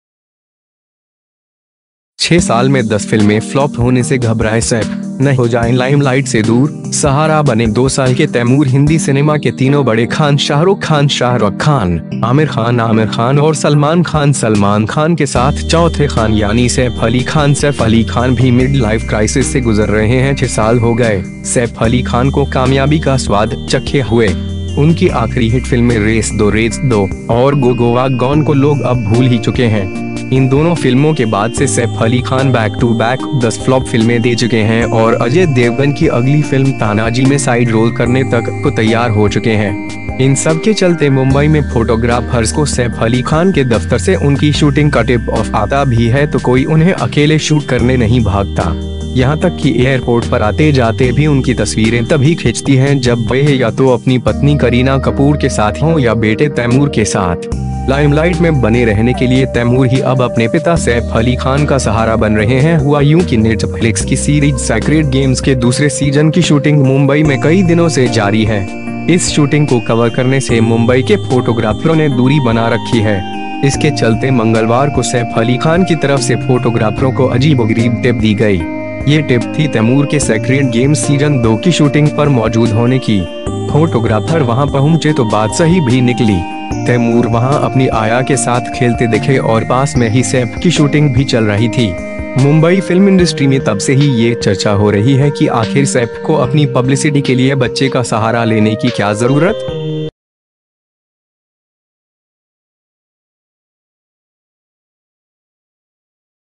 6 साल में 10 फिल्में फ्लॉप होने से घबराए सैफ, न हो जाएं लाइमलाइट से दूर, सहारा बने 2 साल के तैमूर
7 लगातार फ्लॉप देने के बाद इस एक्टर ने शुरू किया ऐसा काम, बड़े-बड़े रईसजादे भी भरते हैं पानी
फिल्म 'स्टाइल' और 'एक्सक्यूज मी' से एक्टर साहिल खान काफी सुर्खियां बटोरी थीं। फिल्म में उनके साथ शरमन जोशी मुख्य भूमिका में थे। शरमन जोशी और साहिल खान की दमदार एक्टिंग ने फिल्म को हिट बना दिया था। उस फिल्म के बाद शरमन तो अपना करियर बनाने के लिए आगे बढ़ गए लेकिन साहिल खान पीछे छूट गए।

साहिल ने बॉलीवुड में एंट्री तो अच्छी की थी लेकिन अब वो गुमनाम एक्टर की गिनती में शामिल हो गए। गौरतलब है कि साहिल खान ने फिल्म 'स्टाइल' के बाद उन्होंने 'रामाः द सेवियर', 'डबल क्रॉस' और 'अलादिन' जैसी कई फिल्में कीं। लेकिन फिल्म की कमजोर कहानी के चलते यह फिल्में बॉक्स-ऑफिस पर लंबा नहीं टिक सकीं। इन फ्लॉप फिल्मों के बाद उन्हें काम मिलना बंद हो गया।

काम ना मिलने के चलते साहिल ने अपना एक जिम खोला है। उनका जिम काफी पॉपुलर भी है। साहिल खन अब एक सुपरस्टार से भी ज्यादा रईसों की जिंदगी जी रहे हैं। वह दुनिया की हर जगह जाकर पार्टी करते हुए दिखाई देते हैं। इंस्टाग्राम पर उनके लाखों में फॉलोअर्स है। उनकी फीटनसे के आज भी लाखों दीवाने हैं। बता दें कि साहिल खान ने बॉलीवुड एक्ट्रेस निगार खान से साल 2004 में शादी की थी, लेकिन अब दोनों अलग हो गए हैं।

निगार ने साहिल खान पर गे होने का आरोप लगाया था। मीडिया रिपोर्ट के मुताबिक, साहिल की किसी लड़के के साथ कॉम्प्रोमाइजिंग पोजीशन में फोटो वायरल हो गई थी। इसके बाद लोगों ने ये मानना शुरू कर दिया कि साहिल गे हैं। साथ ही निगार ने मीडिया के सामने कह दिया कि उनका पति गे है इसलिए उन्होंने तलाक लेने का फैसला किया था।

आपको बता दें कि साहिल खान ने साल 2001 में फिल्म ‘स्टाइल’ से बॉलीवुड में डेब्यू किया था। इसके बाद उन्होंने साल 2013 तक कुल 7 फिल्में की है। लेकिन यह 7वों फिल्में फ्लॉप रही। बैक टू बैक फ्लॉप फिल्में देने के बाद साहिल खान ने खुद के फिल्म इंडस्ट्री से अलग कर लिया।